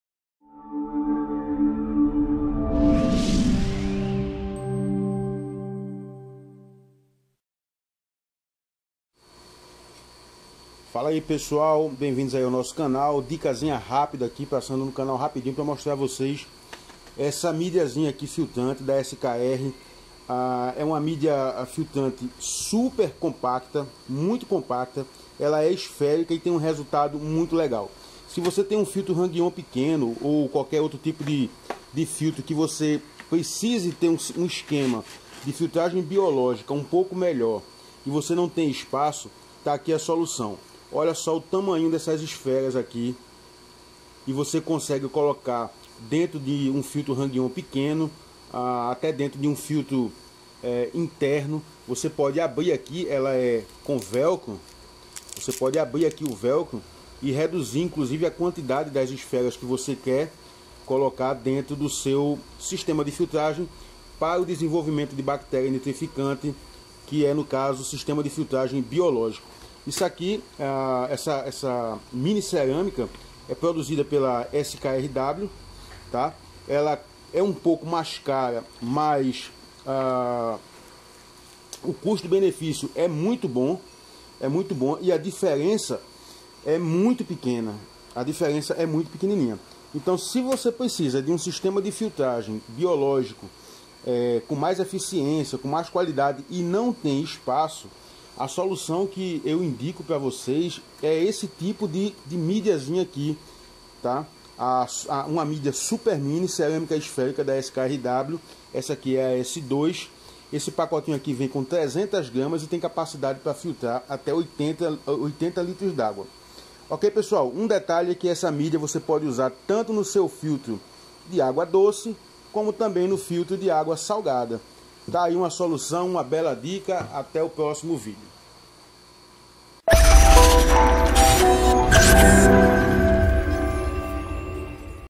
Mini cerâmica SKR S-2 montagem de filtro com reforço biológico.
Descrição
Cerâmica SKRw Esfera Mini S-2 6,8mm Marrom - 300g Sua porosidade prolifera as bactérias necessárias para o cultivo do seu aquário.  A Cerâmica mini branca possuí 1,8mm de diâmetro, proporcionando uma experiência diferenciada pro seu aquário.  Modo de usar: • Colocar dentro do sistema de filtragem de modo que a maior quantidade de água possível passe pela cerâmica.